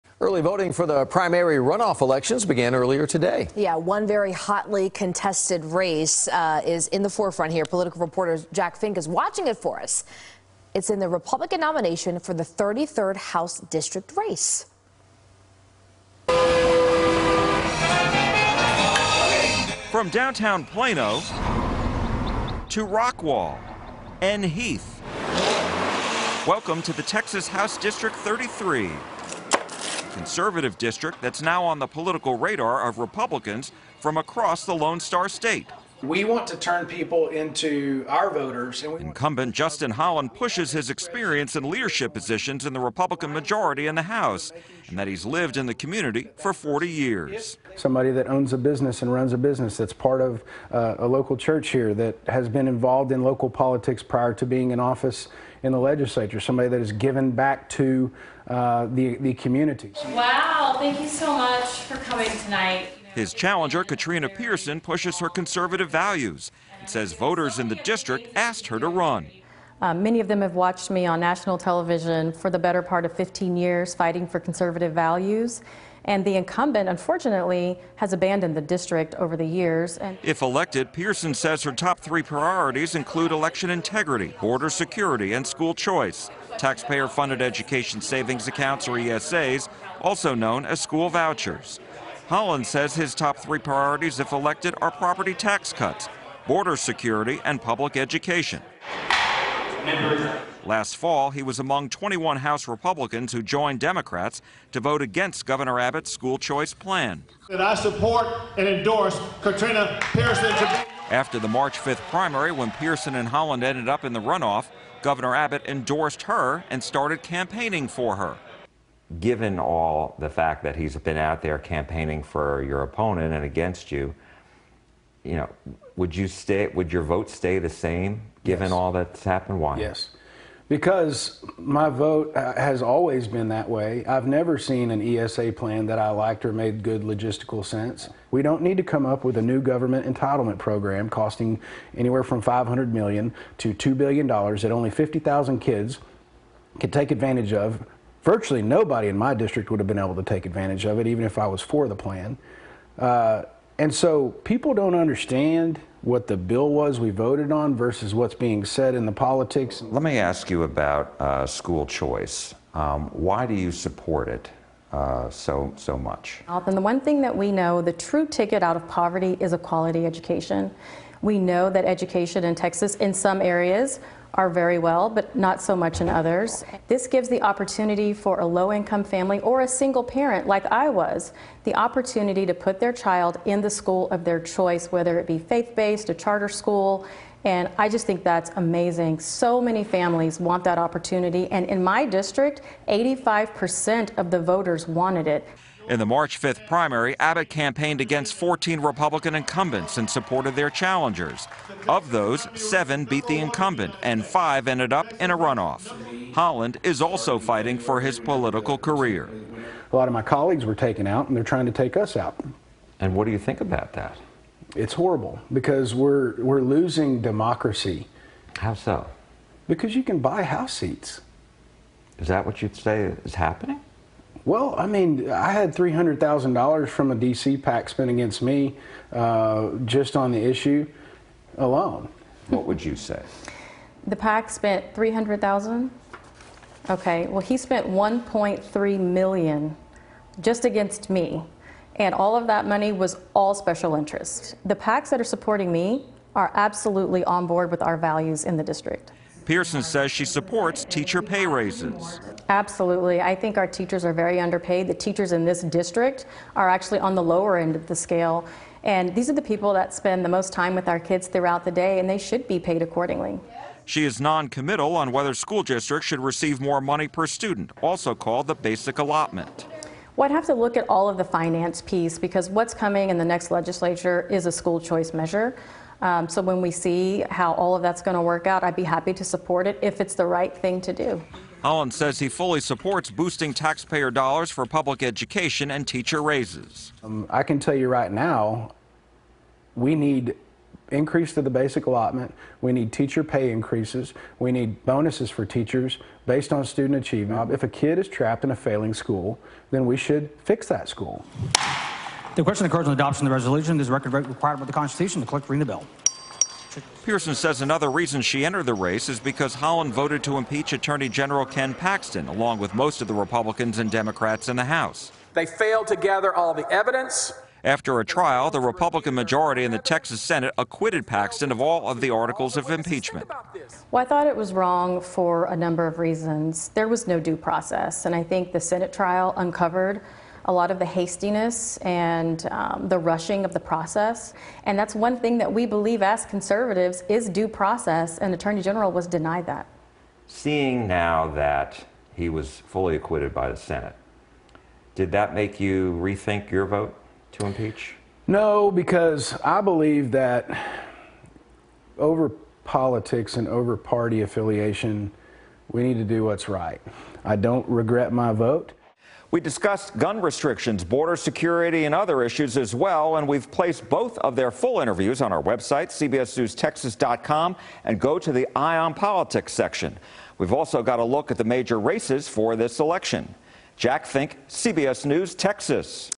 The GOP primary runoff battle for Texas House District 33
Early voting for the primary runoff election is underway.